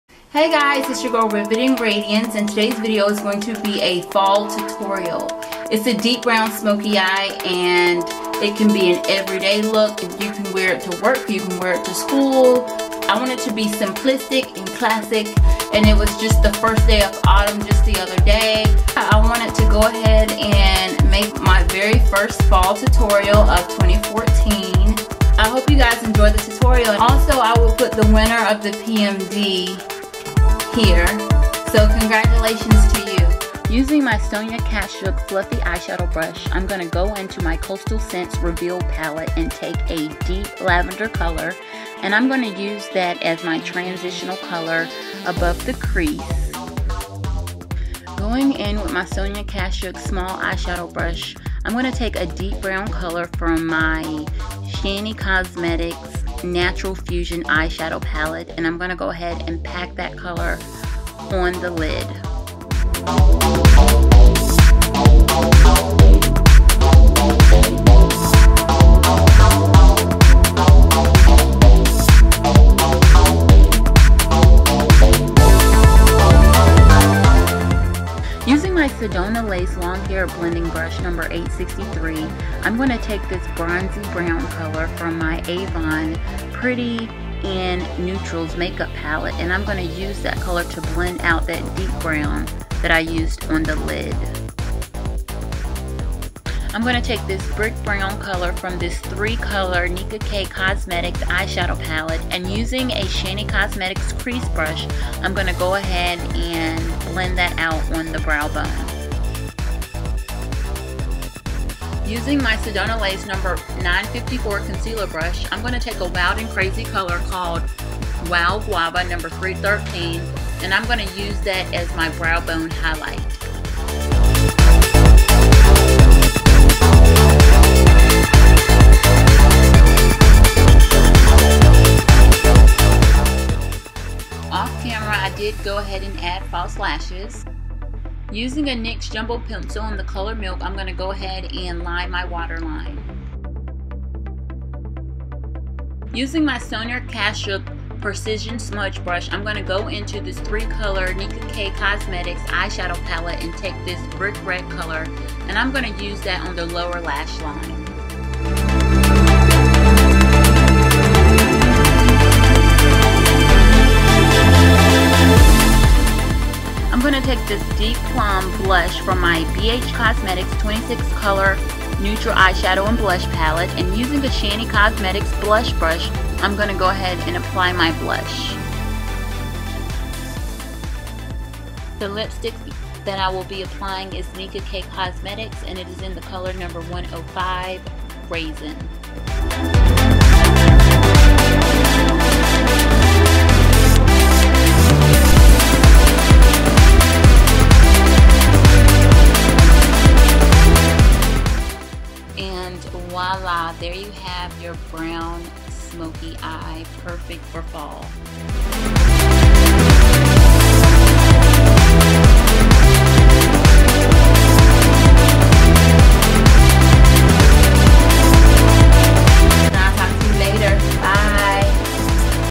EASY! FALL (Brown Smokey Eye) TUTORIAL!
Products Used:

Transition Color : Deep Lavender
Sonia Kashuk Fluffy Eyeshadow Brush :
 Coastal Scents Revealed Palette

Lid Color: Deep Brown
Sonia Kashuk Small Eyeshadow Brush:
Shany Cosmetics Natural Fusion Eyeshadow Palette

Blending out color in crease: Bronze Brown
Sedona Lace Blending Brush #863:
Avon Pretty in Neutrals Make-up Palette

Brown Bone: Reddish Brown Brick color
Shany Cosmetics Crease Crease Brush:
3 color Nicka K Cosmetics Eyeshadow Palette

Brow Highlight: #313 Wild Guava (light salmon color)
Wild & Crazy Cosmetics: Sedona Lace #954 Concealer Brush

Line water Line: NYX Jumbo Pencil : Milk

Line Lower Lash Line: Reddish Brown Brick Color
3 color Nicka K Cosmetics Eyeshadow Palette:
Sonia Kashuk Precision Smudge Brush

Blush: Deep Plum
BH Cosmetics 26 color neutral eyeshadow & Blush palette:
Shany Cosmetics Blush Brush

Lipstick: #105 Rasin:
Nicka K Cosmetics

rivetingradiance